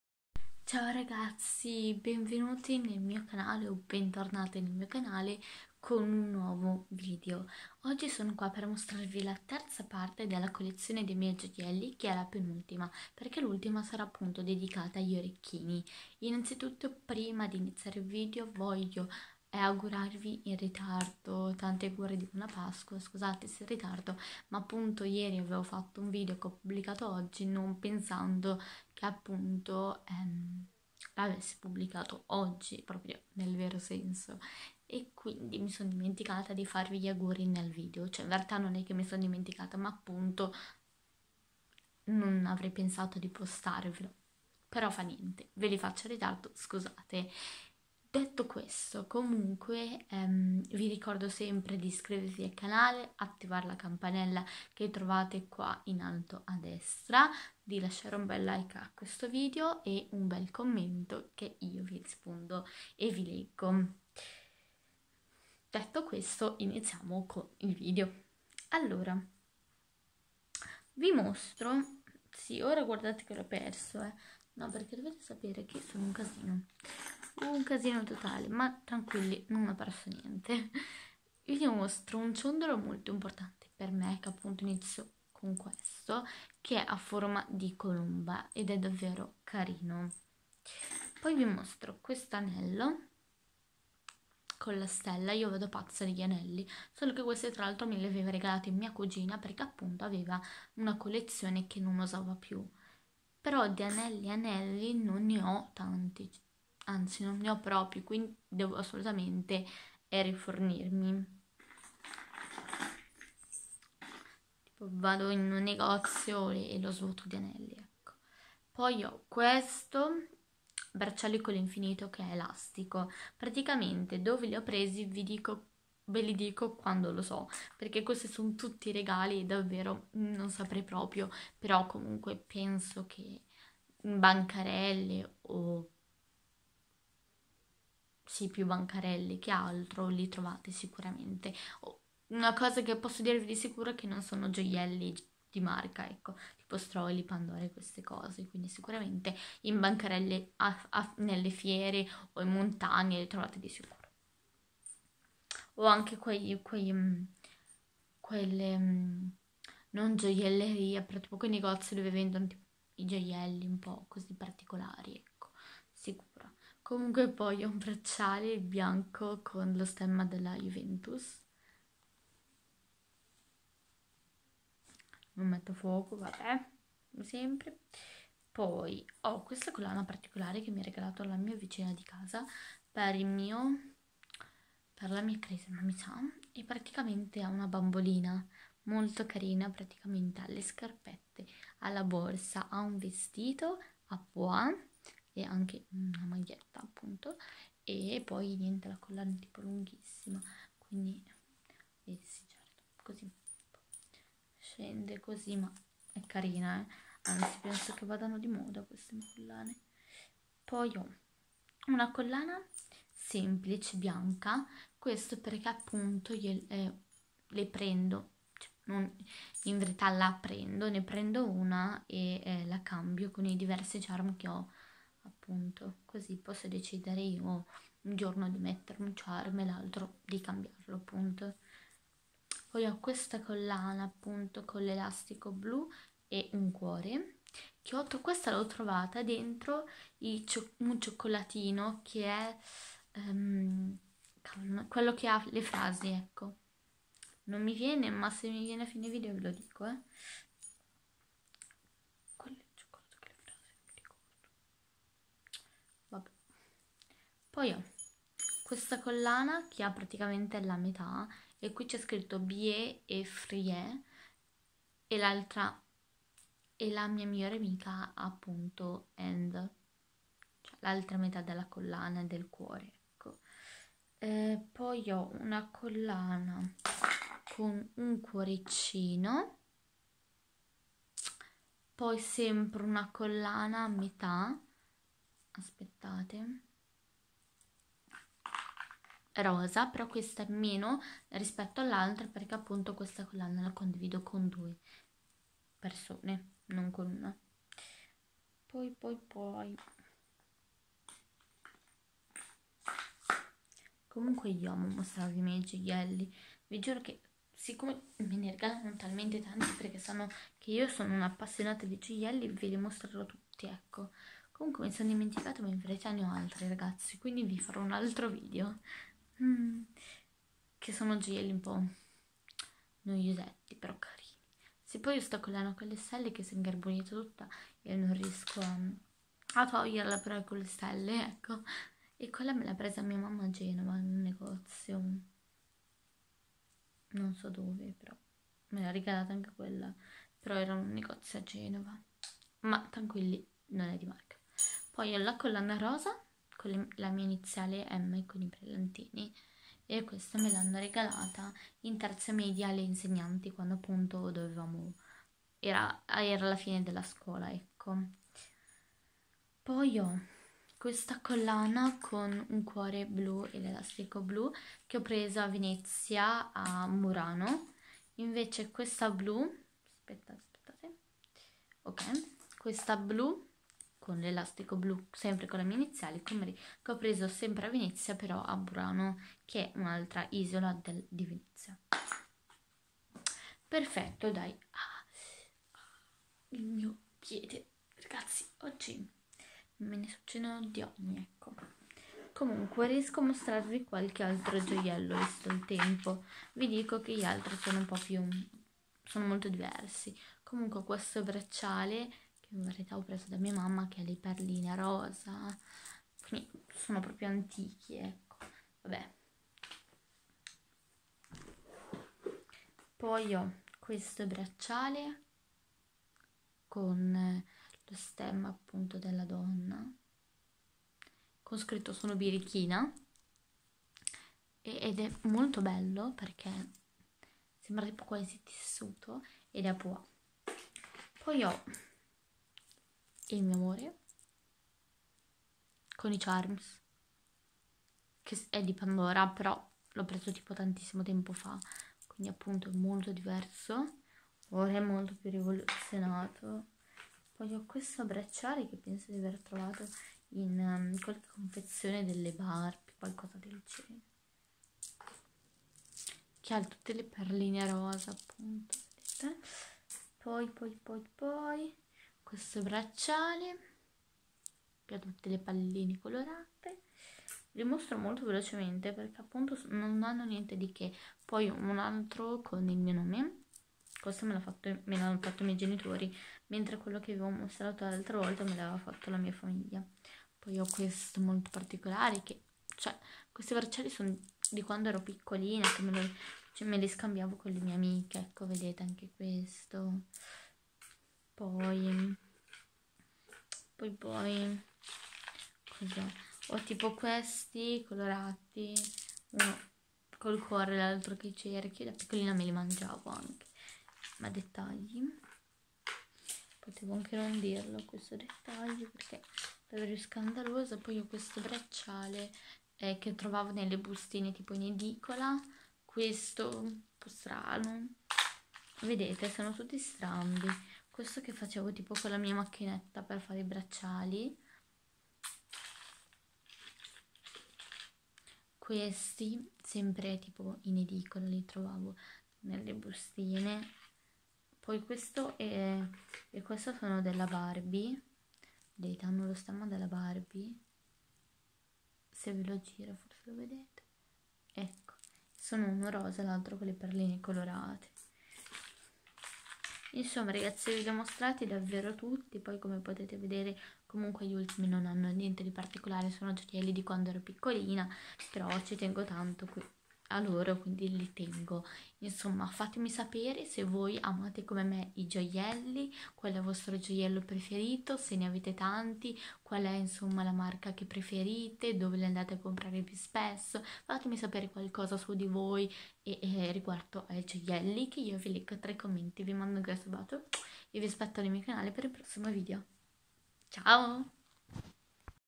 LA COLLEZIONE DEI MIEI GIOIELLI:PARTE 3 |MP🤍
Ciao ragazzi, eccomi con la terza parte della collezione dei miei gioielli, vi prometto che finirò presto 😂ma spero vi piacciano💗avete qualche consiglio su dei video che vorreste vedere nel mio canale? ❤️